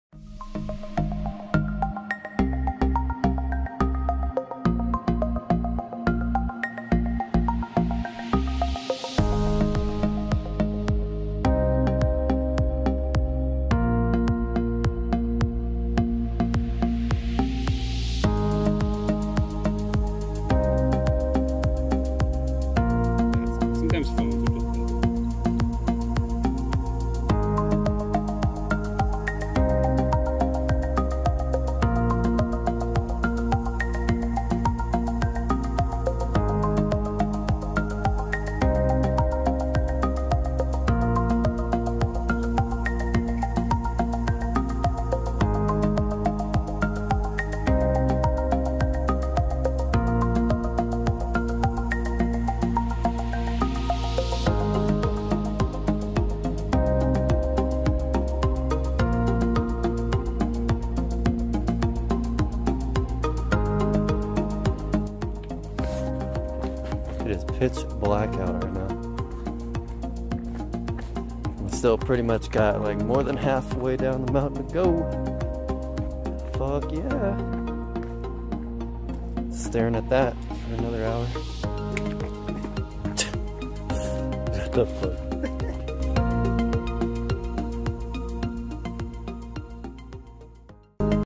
Mt. Si hike for fun!
Me and my buddy Brandon decided on the fly to hike up this big ass mountain.

I wish Windows movie maker was still around :(